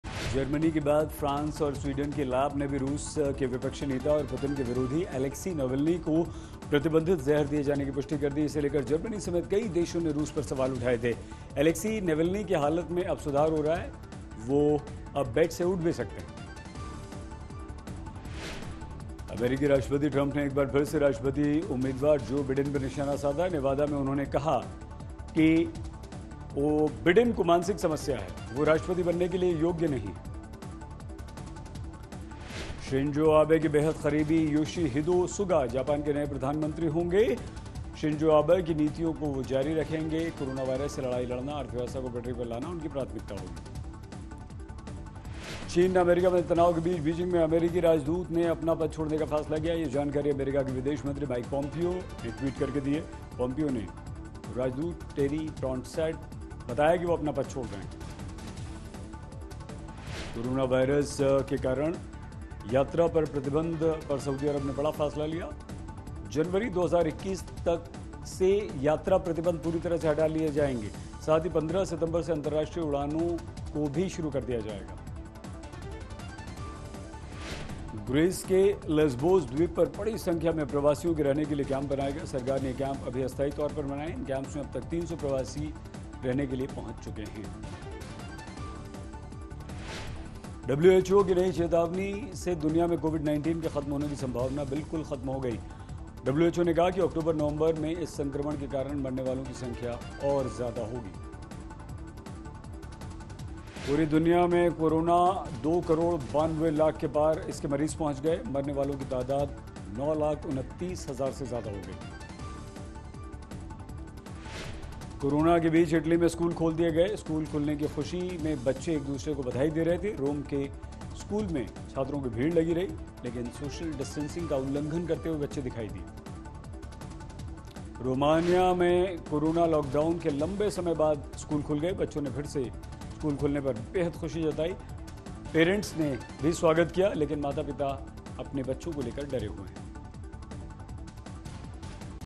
Corona पर WHO की नई चेतावनी- अक्टूबर-नवंबर में और बढ़ेगा मौत का आंकड़ा | Duniya Top 10
विश्व स्वास्थ्य संगठन की नई चेतावनी से दुनिया में कोविड-19 के खत्म होने की संभावना बिल्कुल खत्म हो गई है. WHO  ने कहा है कि अक्टूबर और नवंबर माह में इस संक्रमण के कारण मरने वालों की संख्या और अधिक हो जाएगी. अन्य खबरों के लिए दुनिया टॉप 10 देखिए.